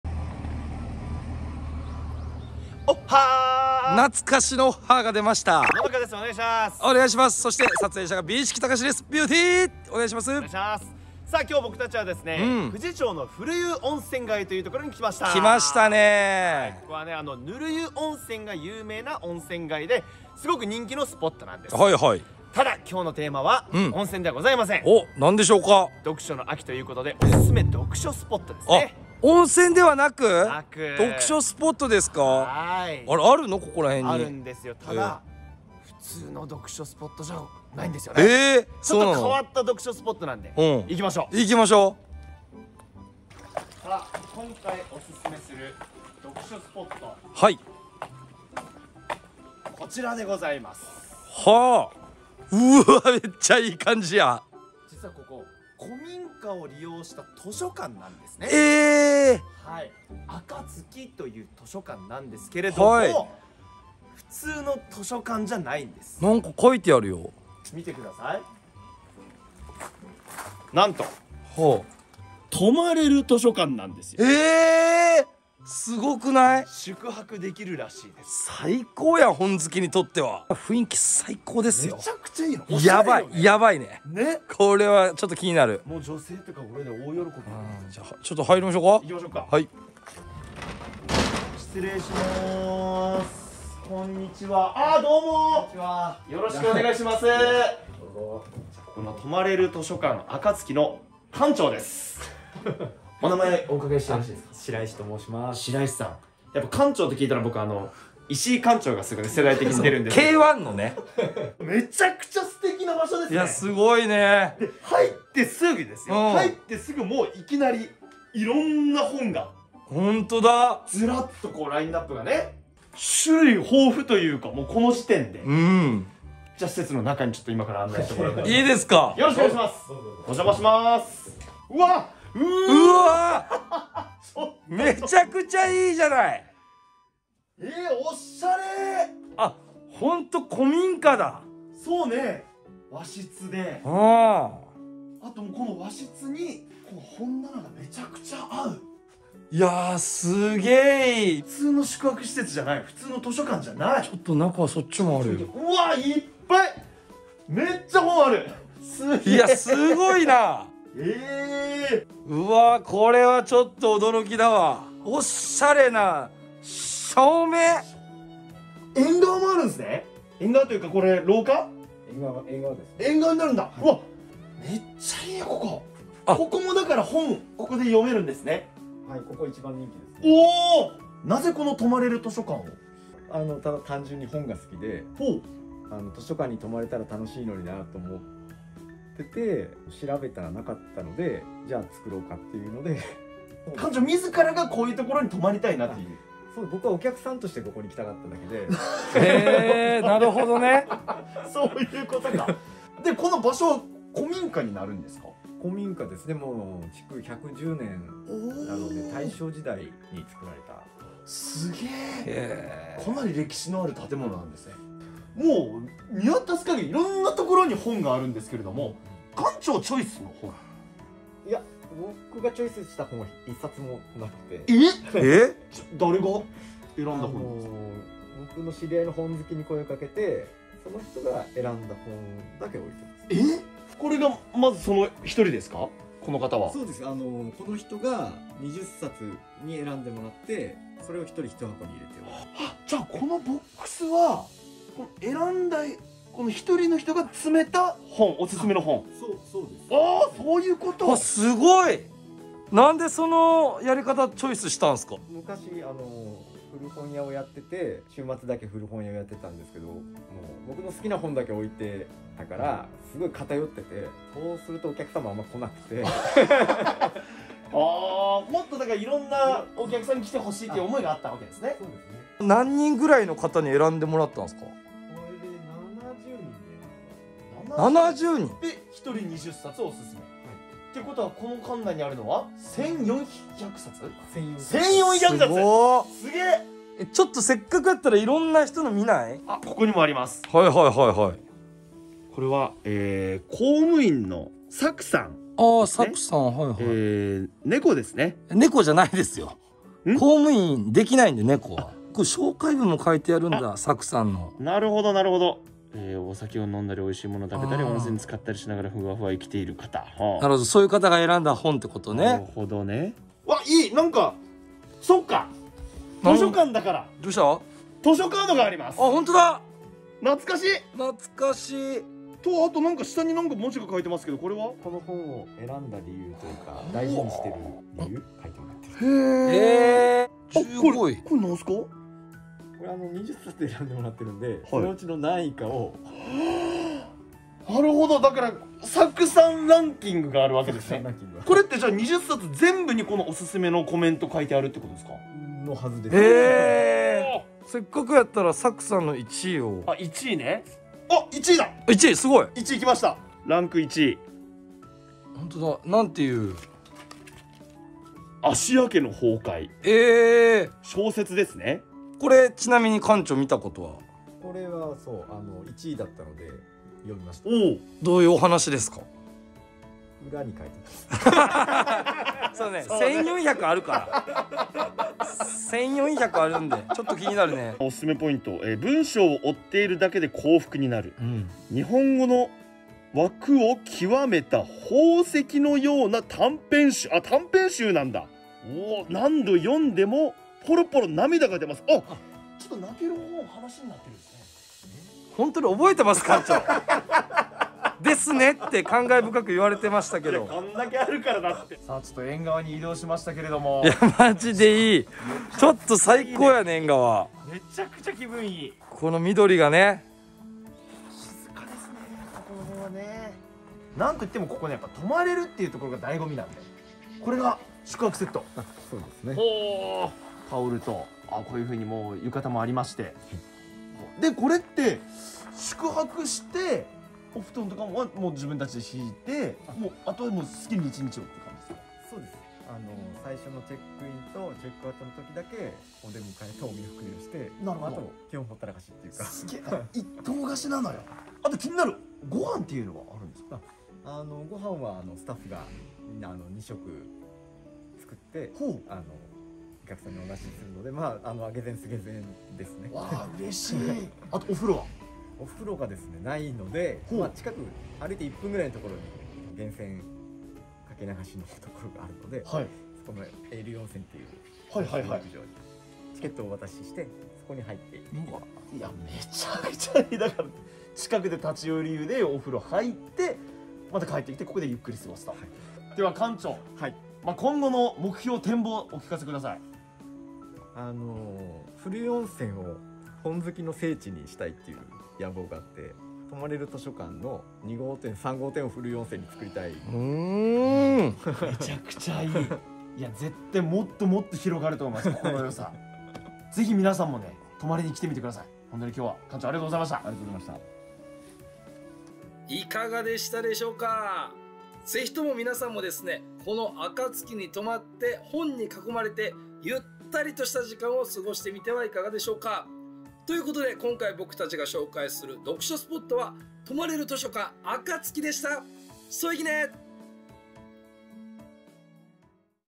【佐賀県】【宿泊】本好きにはたまらない！泊まれる図書館「珈琲 暁」
佐賀県住みます芸人のメタルラックです！

今回は、富士町の古湯温泉街にある、泊まれる図書館「珈琲　暁」さんをご紹介します！

本好きにはたまらない、本に囲まれた1日を過ごすことができます！
気になった本を読みながら景色を見たり、寝落ちしてみたりたっぷりと本を満喫できます！